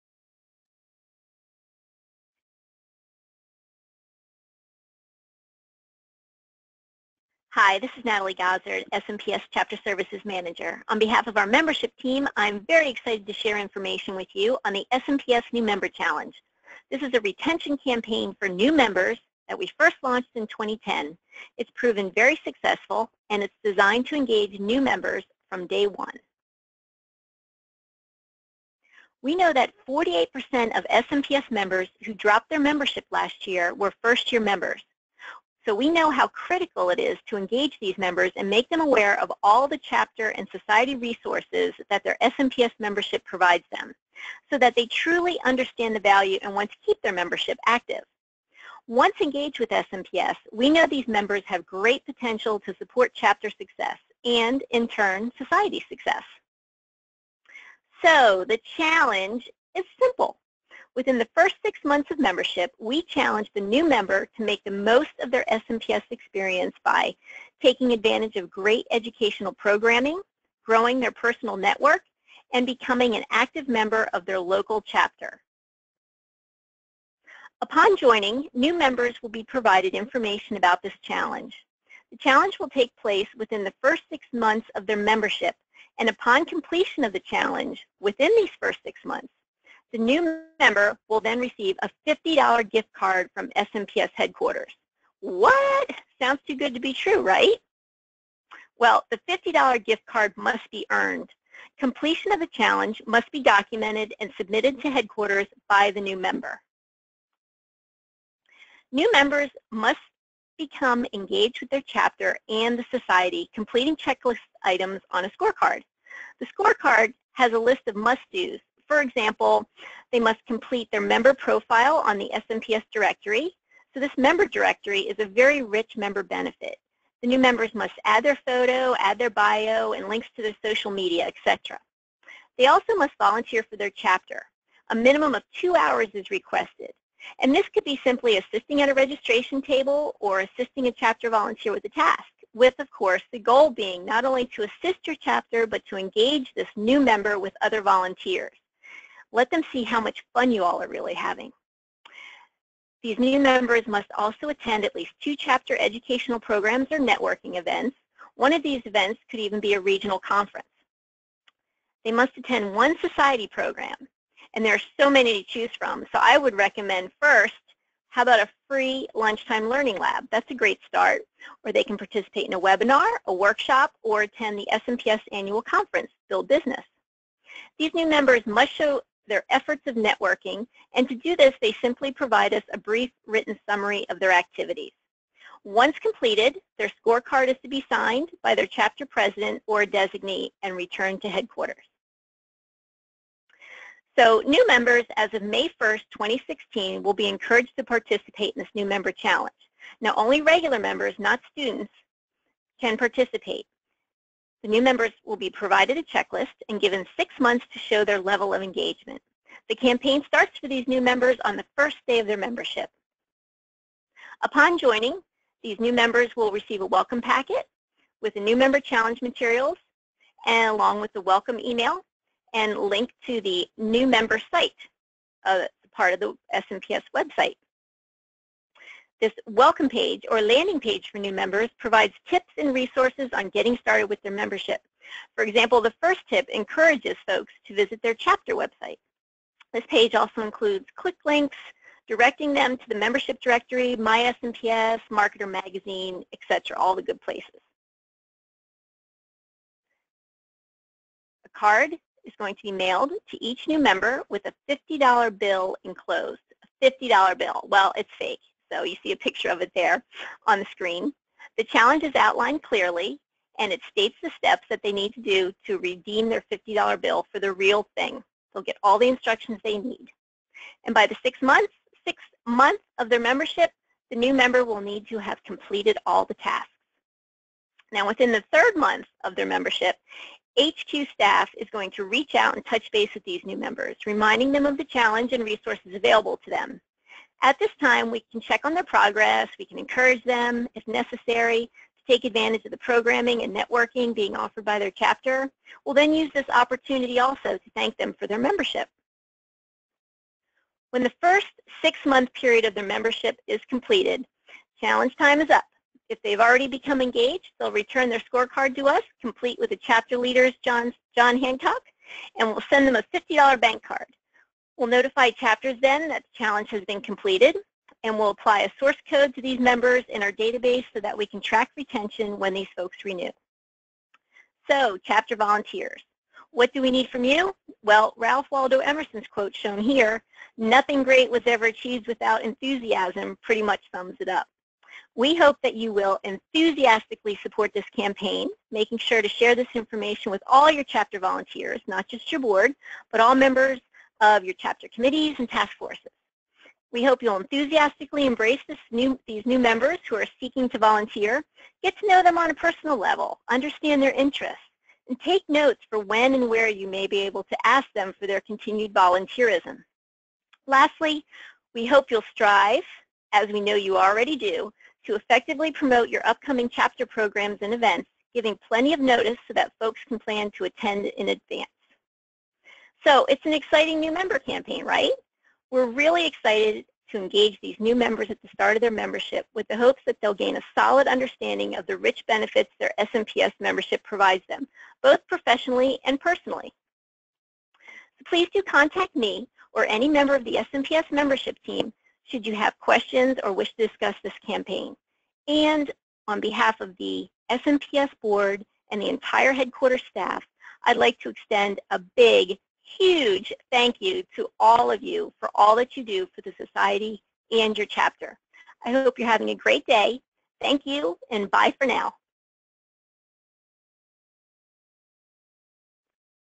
Membership: New Member Challenge
Conducted by SMPS HQ in 2016, informational webinar for chapter membership chairs and other volunteers about the New Member Challenge.